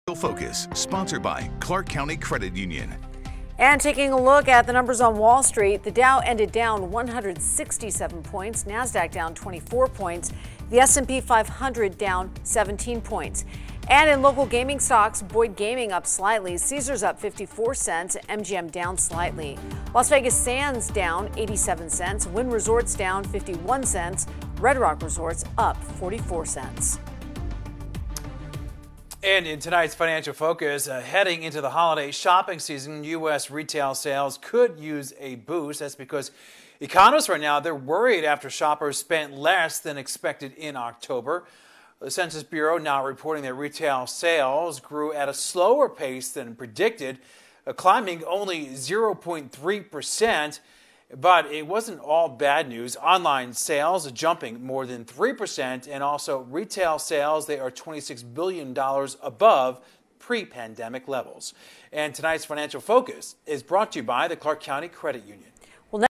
Financial Focus for November 17
Heading into the holiday shopping season US retail sales could use a boost. Economists are worried after shoppers spent less than expected in October.